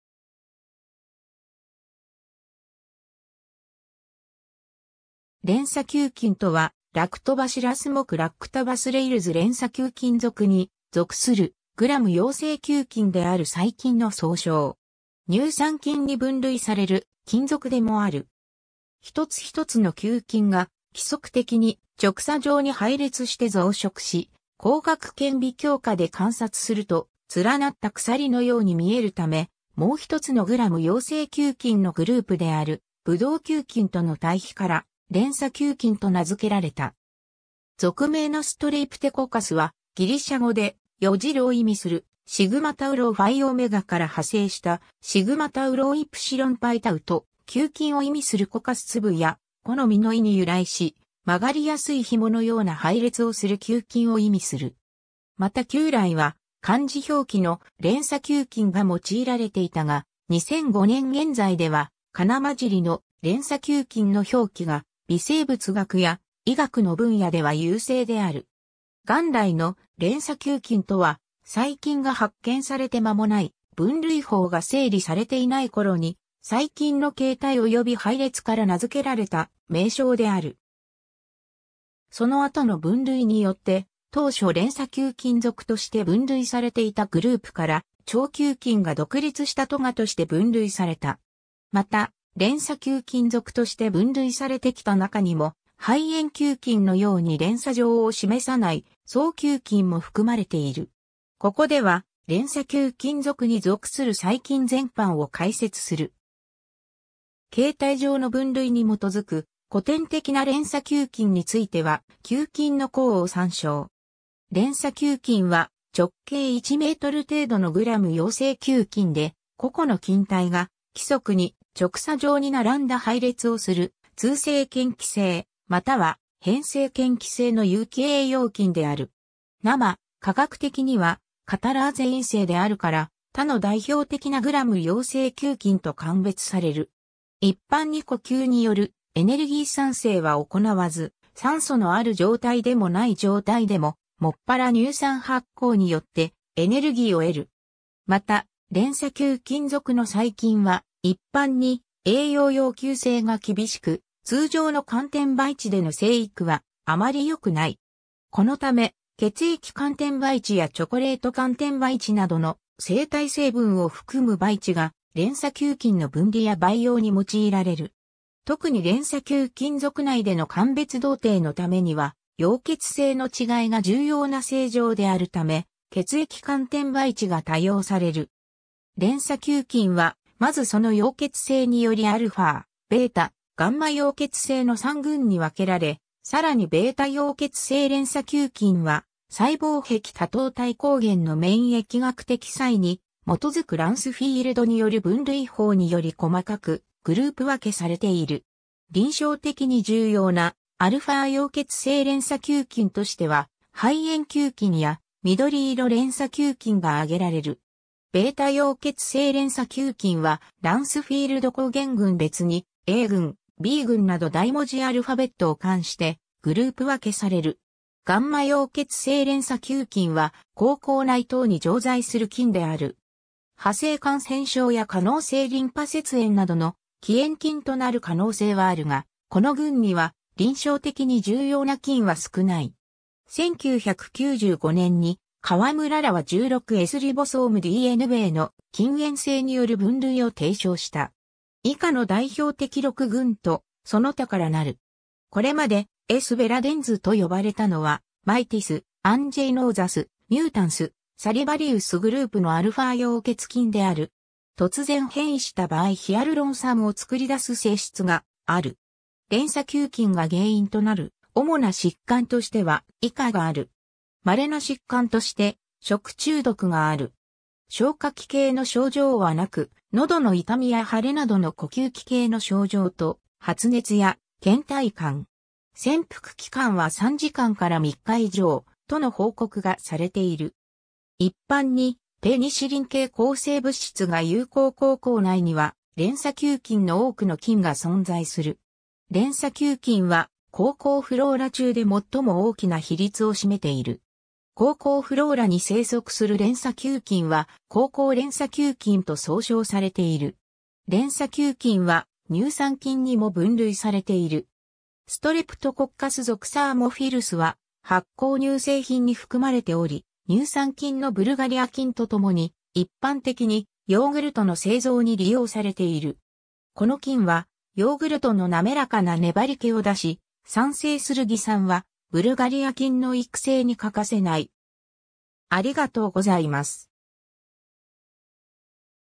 レンサ球菌
レンサ球菌（レンサきゅうきん、連鎖球菌）とは、ラクトバシラス目Lactobacillalesレンサ球菌属（Streptococcus 属）に属するグラム陽性球菌である細菌の総称。
乳酸菌に分類される菌属でもある。
 一つ一つの球菌が規則的に、直鎖状に配列して増殖し、光学顕微鏡下で観察すると「連なった鎖」のように見えるため、もう一つのグラム陽性球菌のグループであるブドウ球菌（ブドウの房状に配列する）との対比から「レンサ（連鎖）球菌」と名付けられた。
属名の Streptococcus は、ギリシャ語で「よじる」を意味する στρέφω から派生した στρεπτός （streptos: 曲げやすい、柔軟な）と、球菌を意味する coccus （元はラテン語で「（穀物の）粒」や「木の実」の意）に由来し、曲がりやすい紐のような配列をする球菌を意味する。
また旧来は漢字表記の「連鎖球菌」が用いられていたが、2005年現在では仮名交じりの「レンサ球菌」の表記が、微生物学や医学の分野では優勢である。
 元来の「レンサ球菌」(streptococcus) とは、細菌が発見されて間もない、分類法が整理されていない頃に細菌の形態および配列から名付けられた名称である。
その後の分類によって、当初レンサ球菌属として分類されていたグループから腸球菌 (Enterococcus) が独立した科 (Enterococcaceae) として分類された。
またレンサ球菌属として分類されてきた中にも、肺炎球菌 (S. pneumoniae) のように連鎖状を示さない双球菌も含まれている。
ここでは、レンサ球菌属に属する細菌全般 (Streptococcus sp.) を解説する。
形態上の分類に基づく古典的なレンサ球菌については球菌の項を参照。
 レンサ球菌は、直径 1µm程度のグラム陽性球菌で、個々の菌体が規則に直鎖状に並んだ配列をする、通性嫌気性または偏性嫌気性の有機栄養菌である。
生化学的には、カタラーゼ陰性である（カタラーゼ酵素を持たないこと）から、他の代表的なグラム陽性球菌と鑑別される。
一般に呼吸によるエネルギー産生は行わず、酸素のある状態でもない状態でも、もっぱら乳酸発酵によってエネルギーを得る。
またレンサ球菌属の細菌は、一般に栄養要求性が厳しく、通常の寒天培地での生育はあまりよくない。
このため血液寒天培地やチョコレート寒天培地などの生体成分を含む培地がレンサ球菌の分離や培養に用いられる。
特にレンサ球菌属内での鑑別同定のためには、溶血性の違いが重要な性状であるため、血液寒天培地が多用される。
 レンサ球菌は、まずその溶血性によりα、β、γ溶血性の3群に分けられ、さらにβ溶血性レンサ球菌は、細胞壁多糖体抗原の免疫学的差異に基づくランスフィールドによる分類法により細かくグループ分けされている。
 臨床的に重要なα溶血性レンサ球菌としては、肺炎球菌（羅 Streptococcus pneumoniae、英では通称 Pneumococcus）や、緑色連鎖球菌（羅 Streptococcus viridans）が挙げられる。
 β溶血性レンサ球菌は、ランスフィールド抗原群別に、A群、B群など大文字アルファベットを冠してグループ分けされる。
 γ溶血性レンサ球菌は、口腔内等に常在する菌である。
歯性感染症や化膿性リンパ節炎などの起炎菌となる可能性はあるが、この群には臨床的に重要な菌は少ない。
 1995年にKawamuraらは16SリボソームDNAの近縁性による分類を提唱した。
以下の代表的6群とその他からなる。
これまで S. viridans と呼ばれたのは、Mitis, Anginosus, Mutans, Salivarius groupのα溶血菌である。
 突然変異した場合ヒアルロン酸を作り出す性質がある。
 レンサ球菌が原因となる主な疾患としては以下がある。
 希な疾患として食中毒がある。
消化器系の症状は無く、喉の痛みやはれなどの呼吸器系の症状と発熱や倦怠感。
潜伏期間は3時間から3日以上、との報告がされている。
 一般にペニシリン系抗生物質が有効 口腔内にはレンサ球菌（ストレプトッコカス属）の多くの菌が存在する。
レンサ球菌は口腔フローラ中で最も大きな比率を占めている。
口腔フローラに生息するレンサ球菌は口腔レンサ球菌 (oral streptococci) と総称されている。
 レンサ球菌は乳酸菌にも分類されている。
ストレプトコッカス属サーモフィルス(Str...